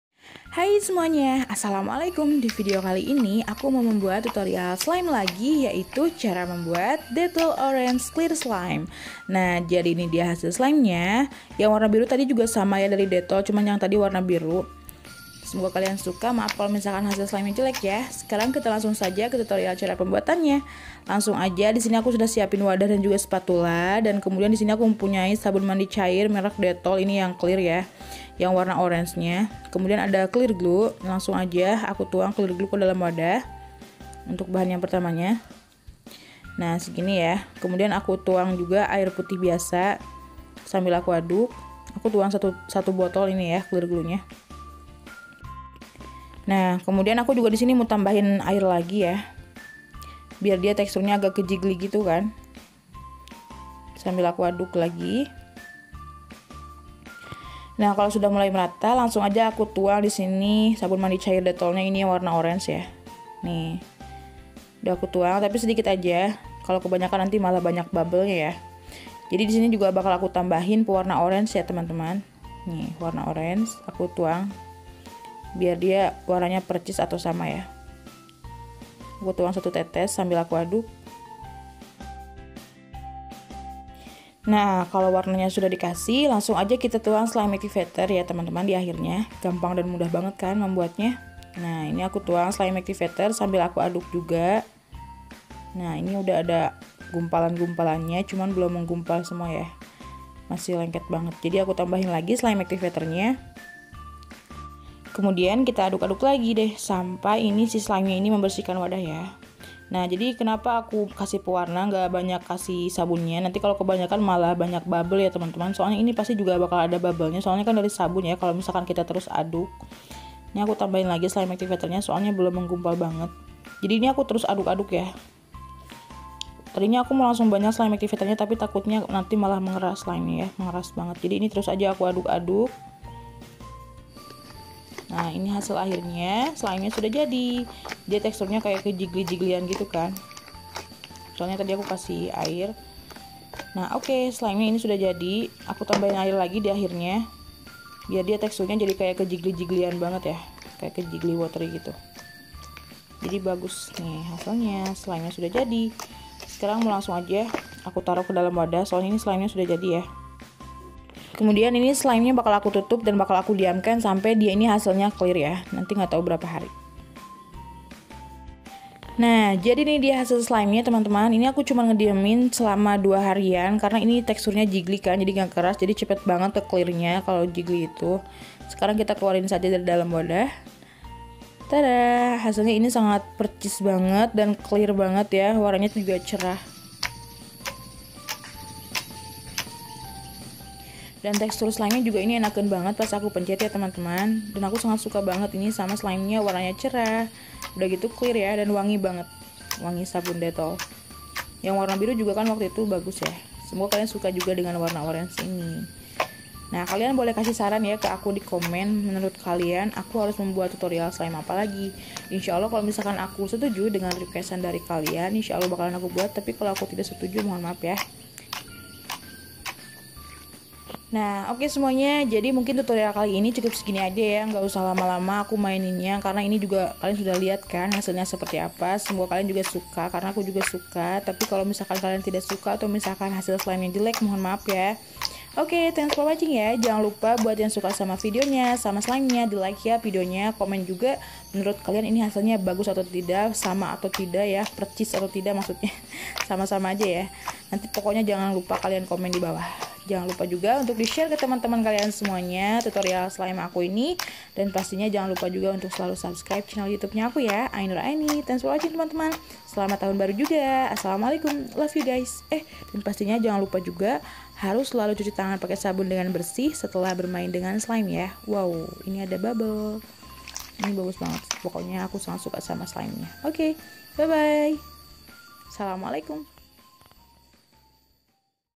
Cara membuat slime Dettol Orange Clear slime
Hai semuanya jadi di video kali ini aku mau membuat tutorial slime lagi yaitu cara membuat slime dari sabun dettol yang warna orange.
semoga kalian suka ya dengan hasil slime nya.

maaf kalau misalkan jelek.

Love you😘😍🤩